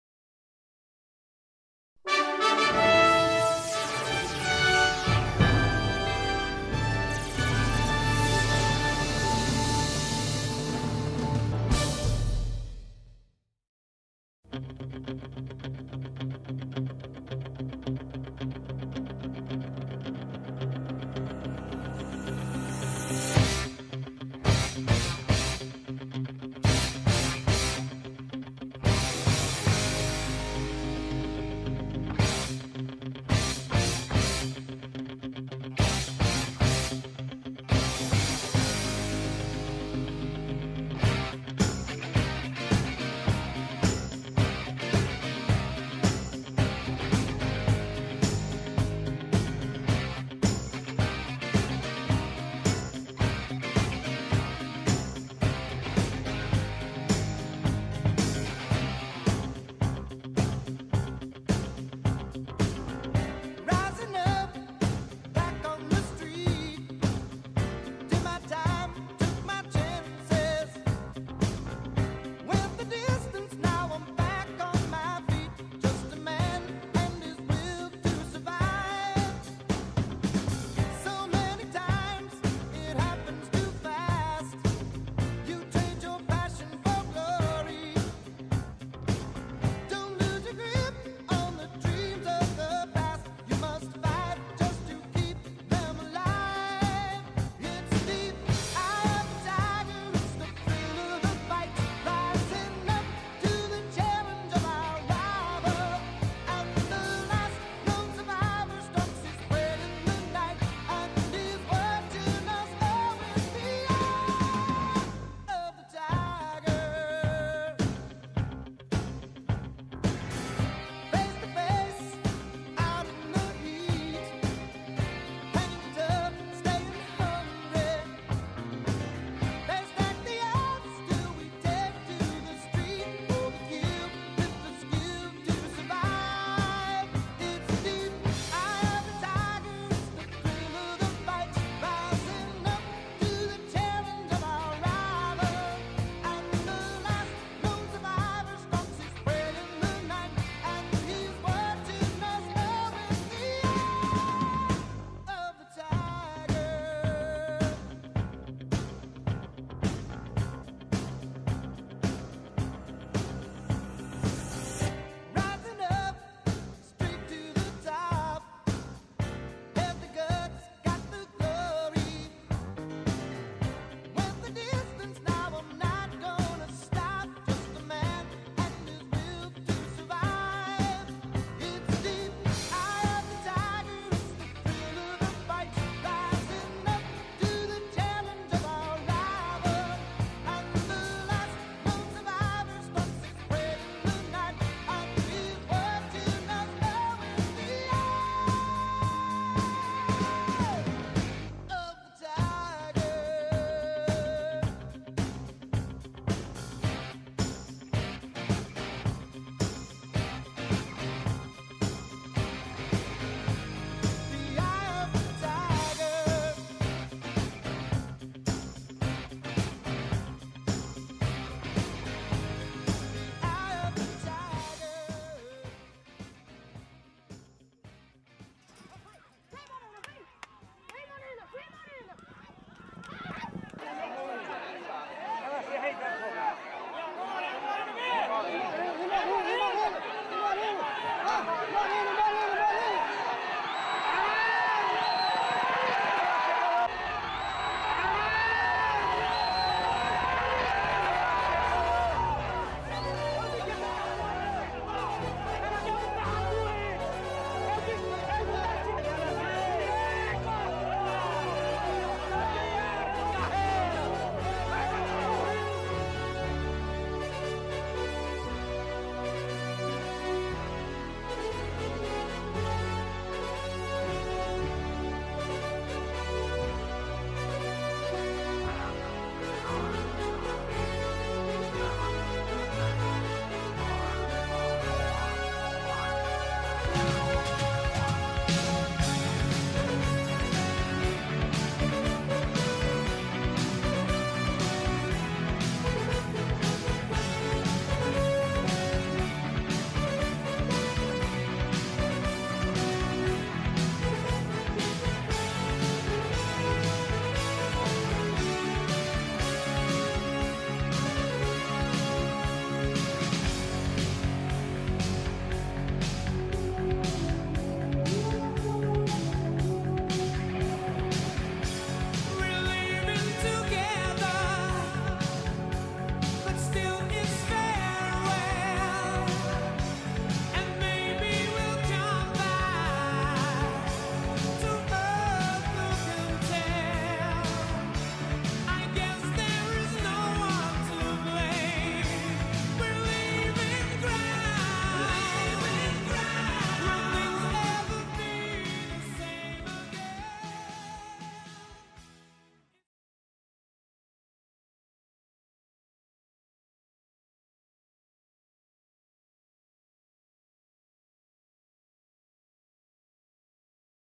Carreira do Ano - MORENA X MELANCIA - Guaraciaba do Norte
🛍️ 👉Ofertas do Dia AMAZON
📣 Arrase dos Pés à Cabeça: Descubra a Moda Feminina Perfeita para Você aqui!

Grande carreira de cavalo entre Morena e Melancia. Hipódromo de Guaraciaba do Norte.